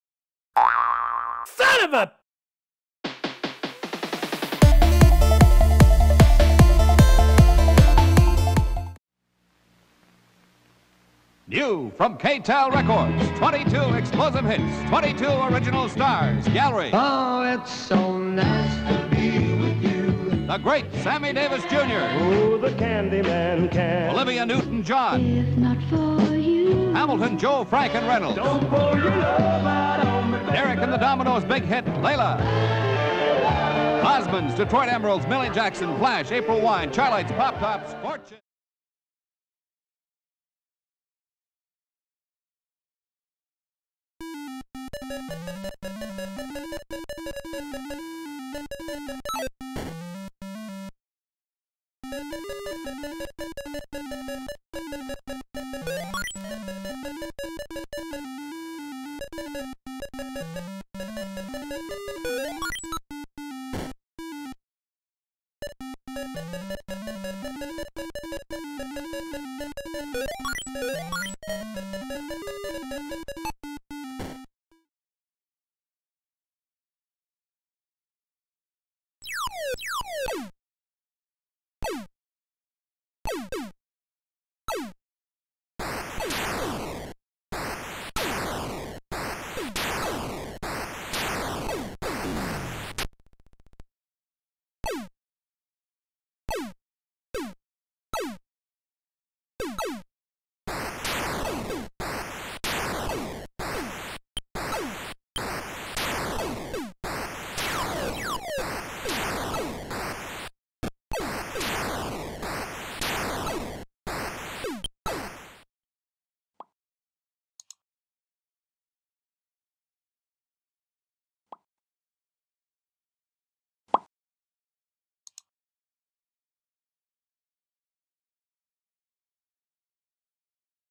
When K-Tel Video Games Roamed the Earth
K-Tel is known for selling questionable kitchen gadgets, fishing gadgets, "20 Greatest Hits" collections, and other "As Seen on TV" products. But did you know they tried their hand at publishing games for the Atari 2600?